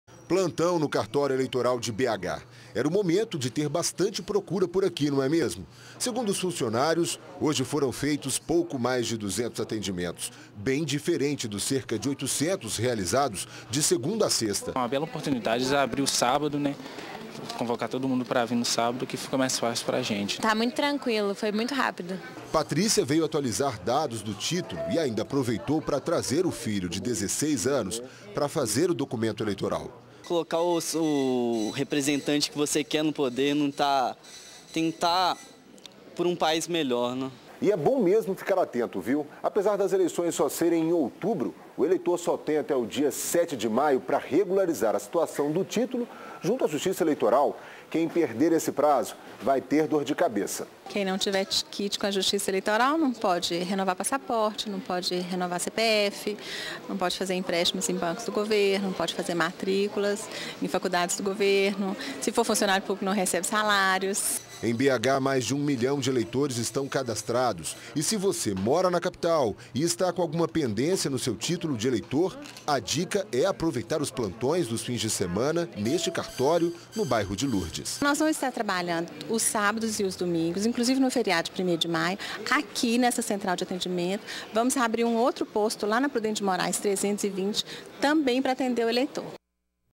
Cartórios do TRE funcionam em regime de plantão nos finais de semana e feriado
O Tribunal Regional Eleitoral (TRE) de Minas Gerais começou neste sábado plantões nos cartórios para atender eleitores que desejam fazer a inscrição ou atualização do título para as eleições deste ano.